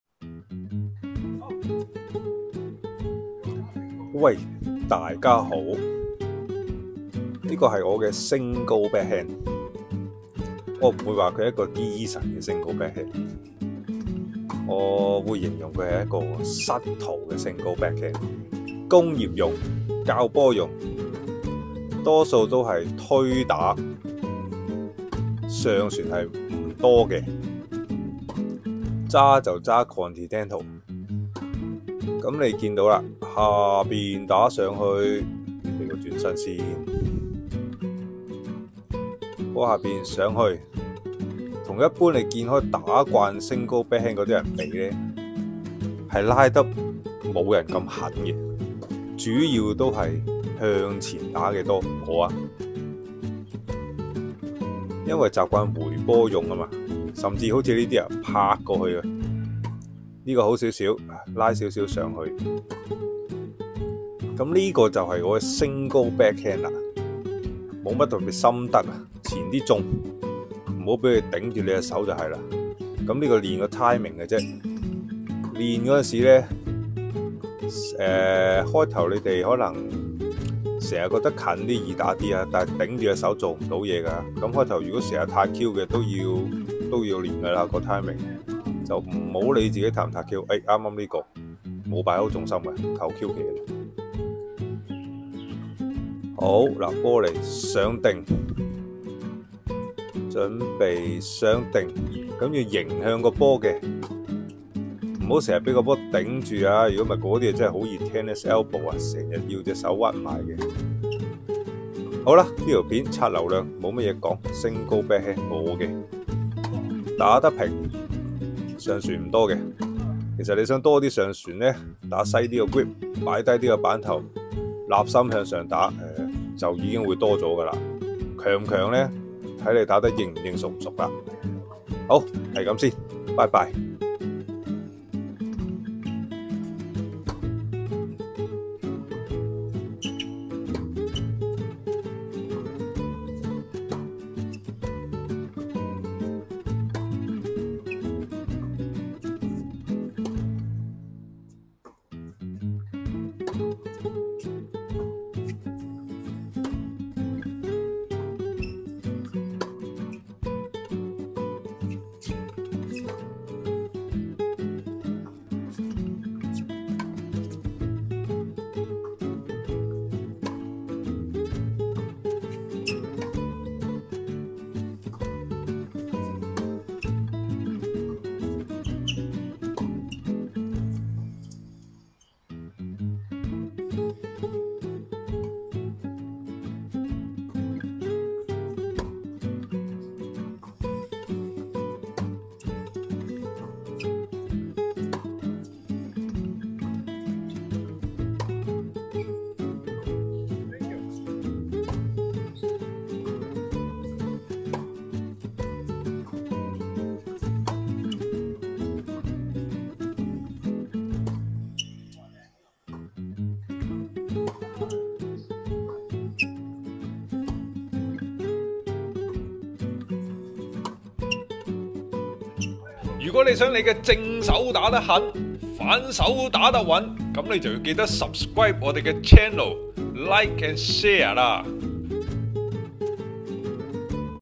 吾想打網球🎾 一個subtle的單反
一個subtle 嘅單反，開工用，善良的的反手😛

因為訂閱人數唔夠多,有啲工能未用到,開多個IG出吓POST.得閒可以過去睇下.

黃師傅Wechat ID: Stanley22336688